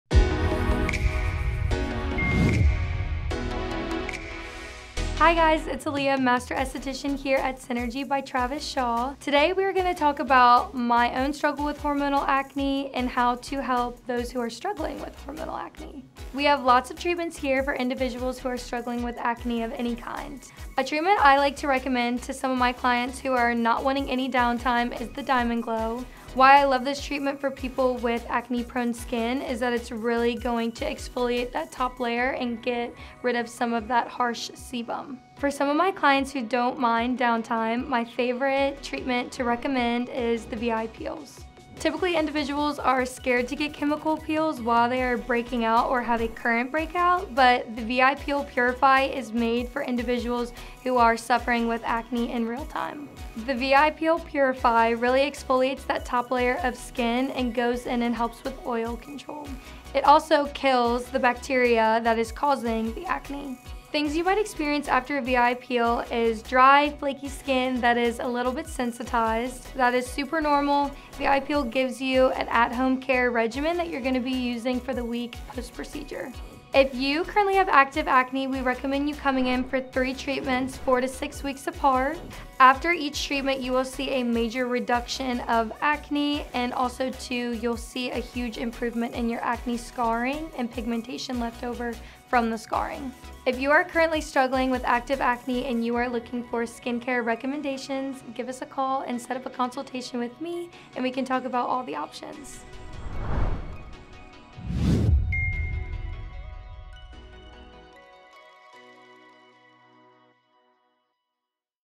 Combat Hormonal Acne with the VI Peel | with Aliyah Bowman
If you are struggling with acne, you are NOT alone!

We are here to help! We have many different techniques that you can try - like a Diamond Glow treatment or the VI Peel. The VI Peel is a great treatment that will leave you with more confidence that is goes deeper than just skin! ✨